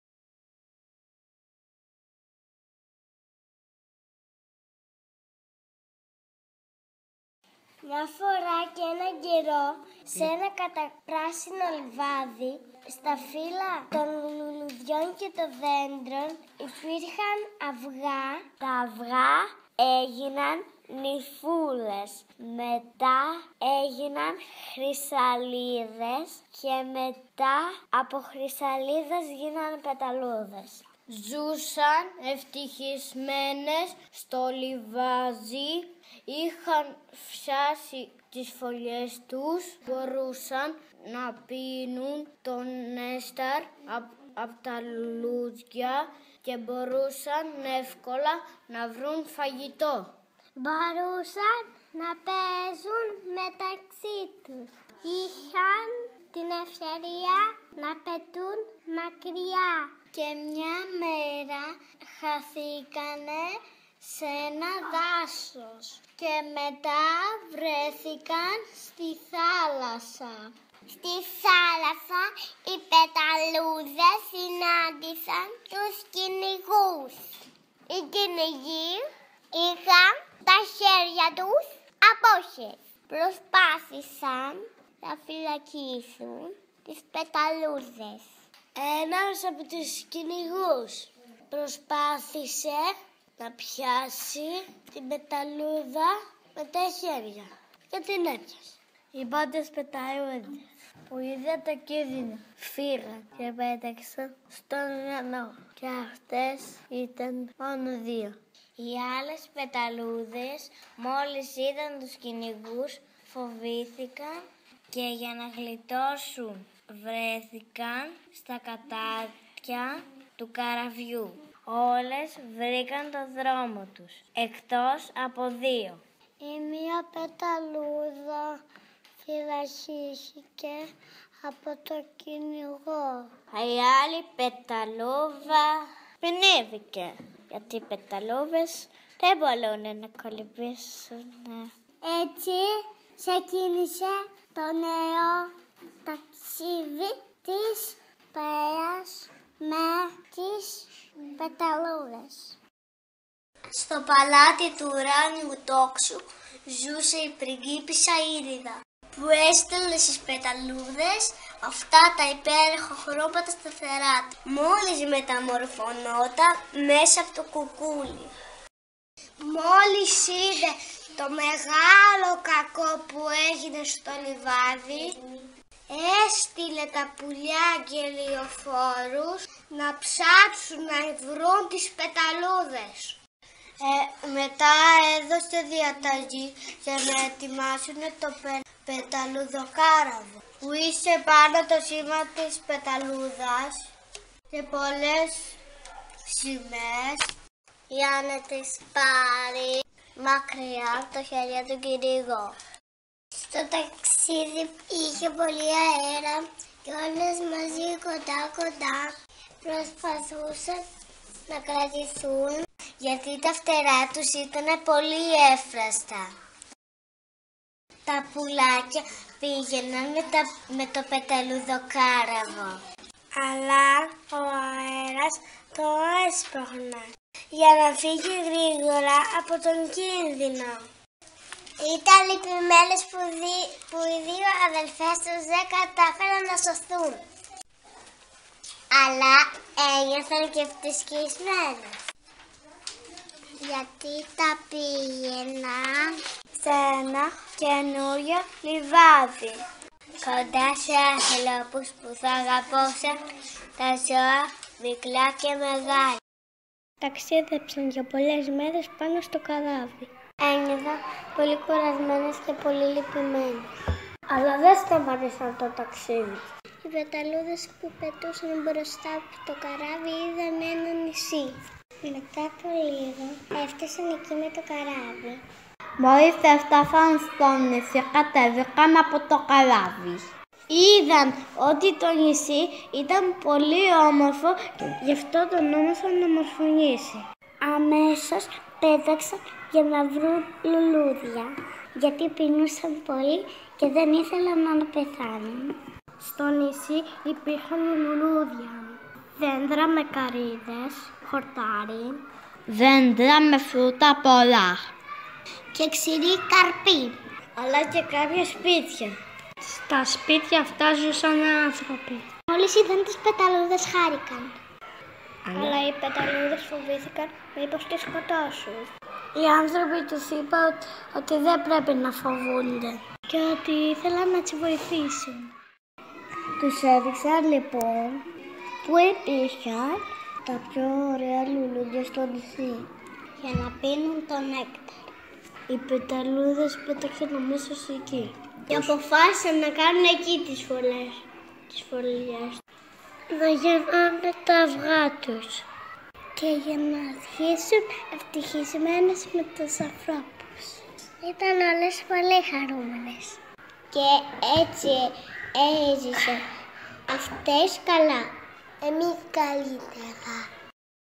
The butterfly ship
A story created in common by all the pupils participating in the eTwinning project "Art... attack!!!".